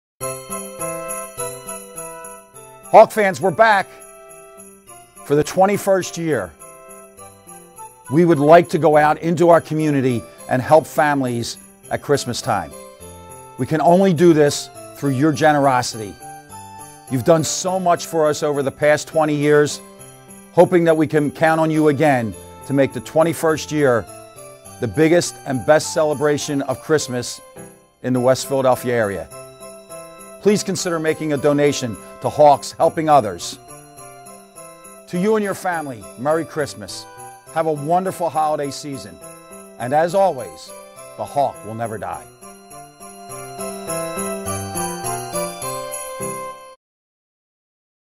Saint Joseph's University Men's Basketball Christmas Charity
Help the Hawks brighten the Christmas holidays for families in West Philadelphia.  Contributions can be made in the Hagan Arena lobby